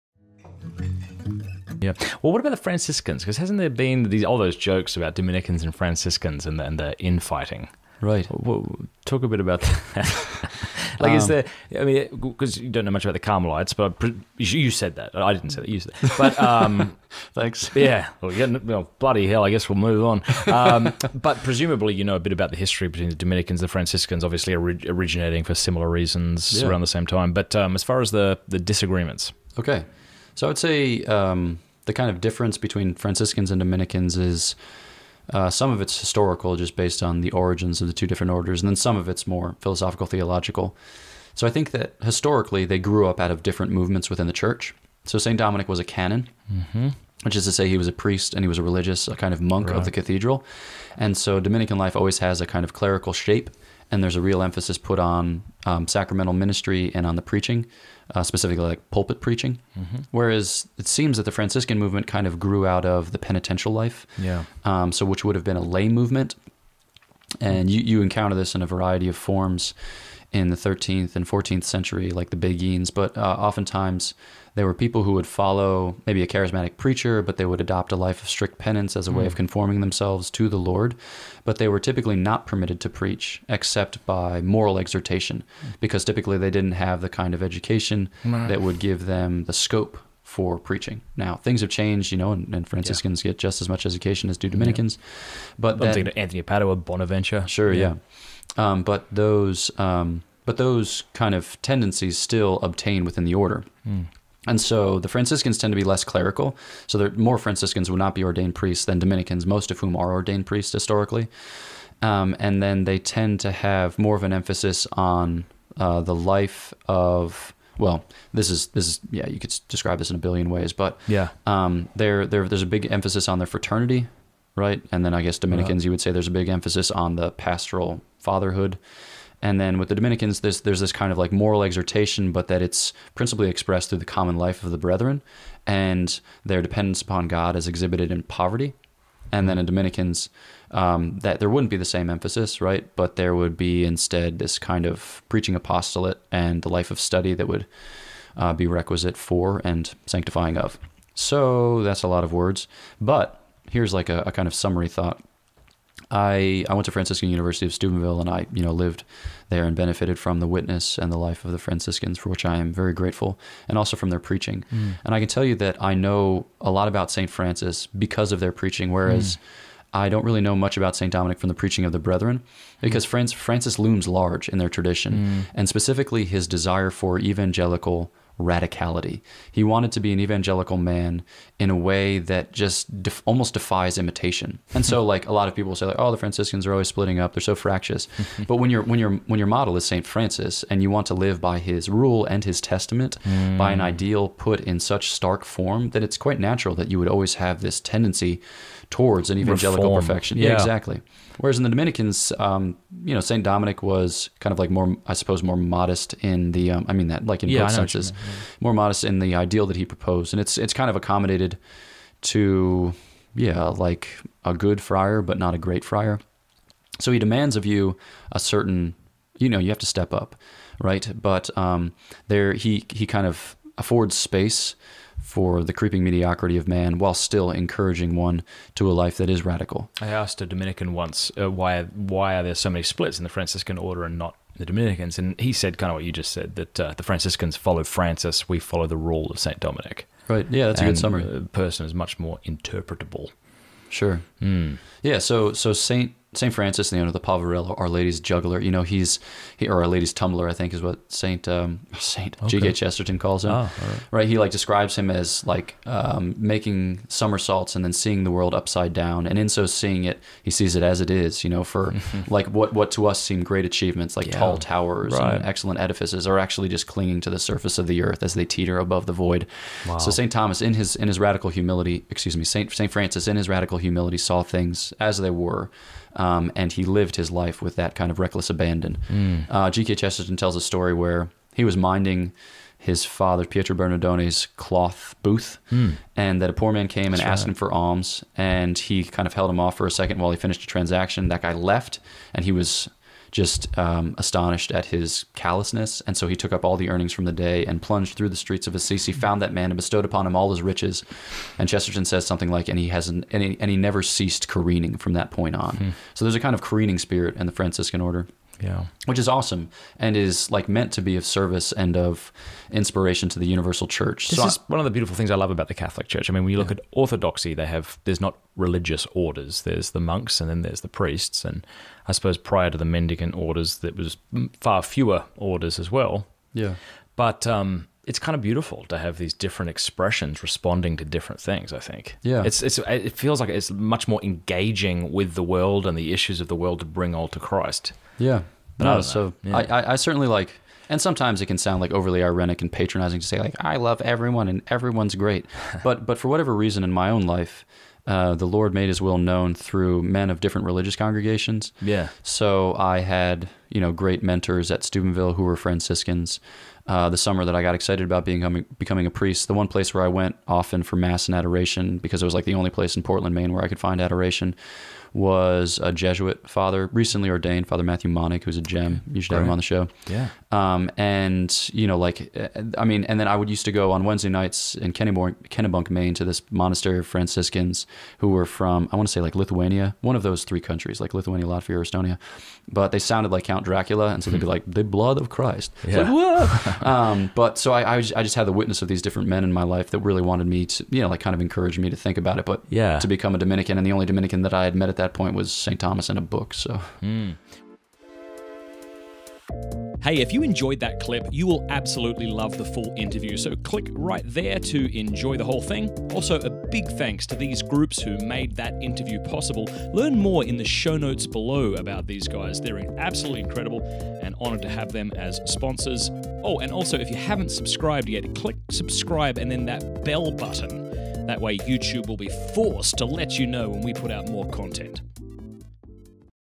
The Real Difference Between Franciscans and Dominicans W/ Fr. Gregory Pine
Fr. Pine shares his take on the differences and similarities between the Franciscan order and his own.

📘 We Wrote a Book Together on Marian Consecration!